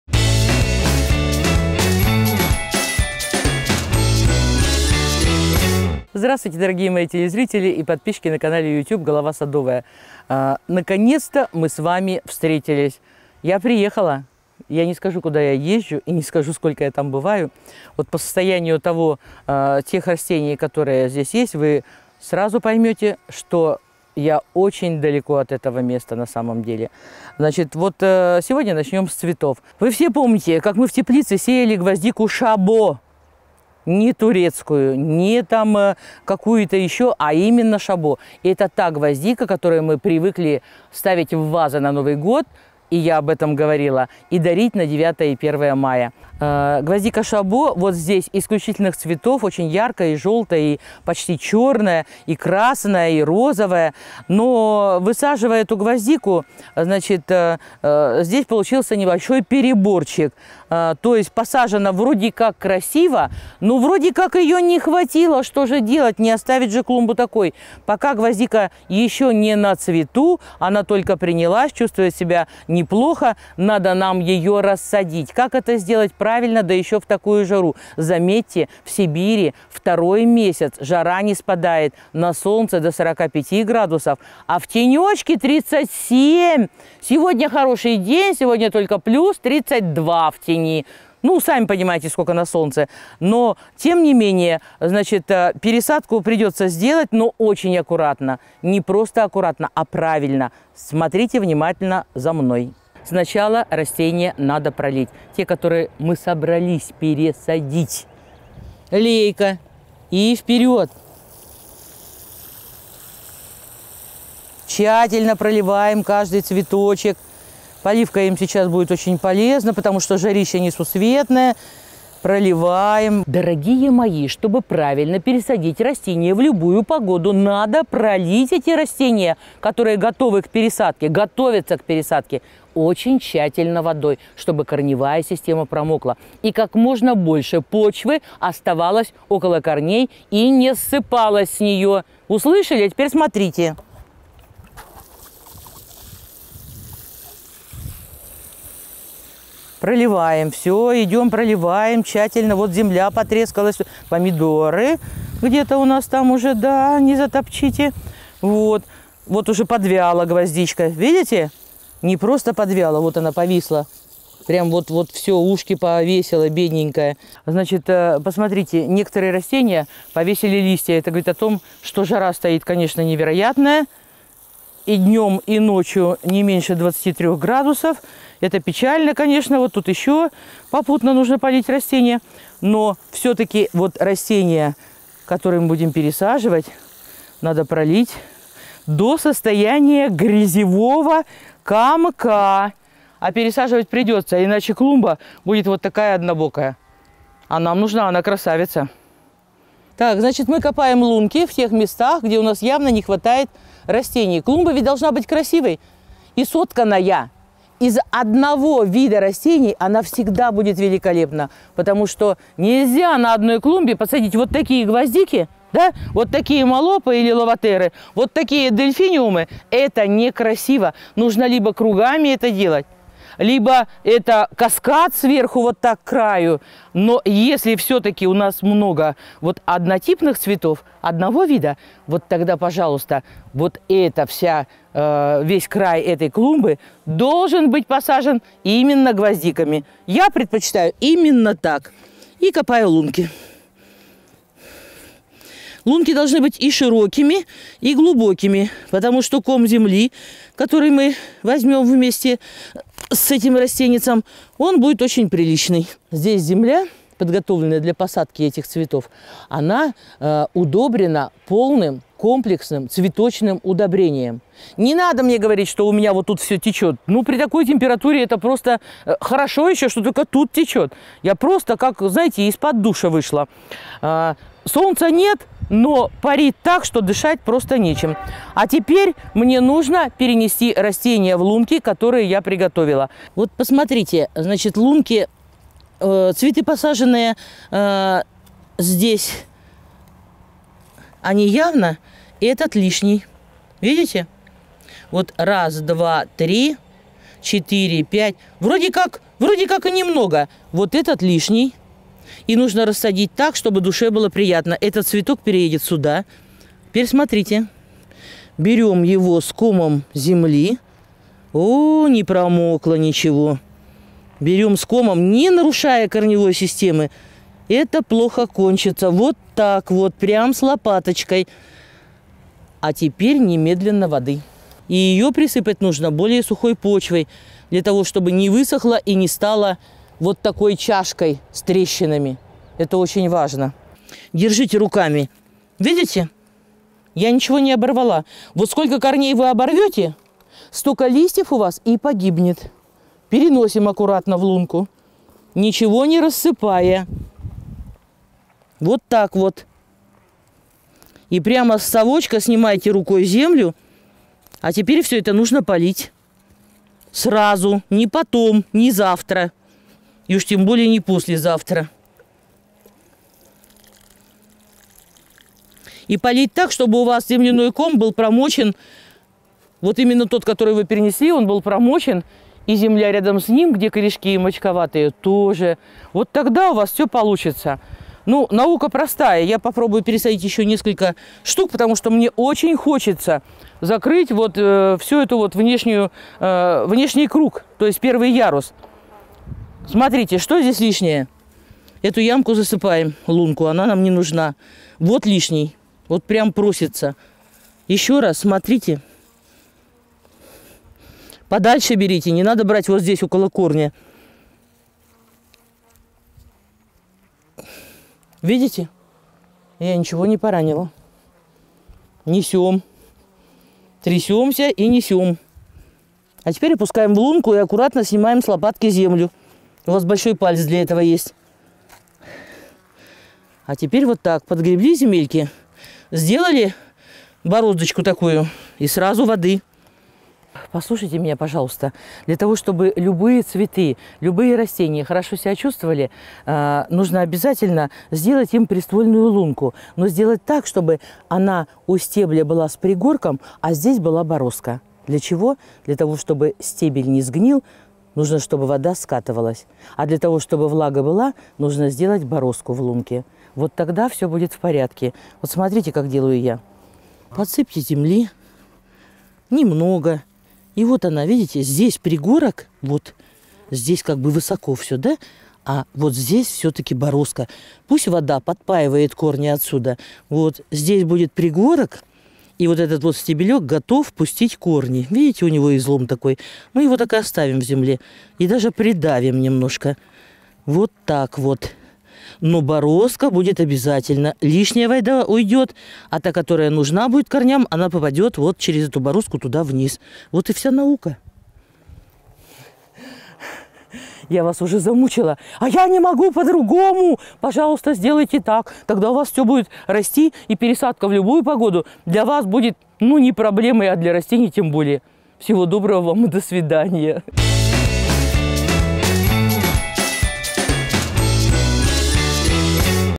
Голова садовая - Как пересадить рассаду гвоздики Шабо
Сегодня поговорим, как пересадить рассаду гвоздики Шабо. Какие тонкости, что учесть и как обеспечить себе красивейшую клумбу!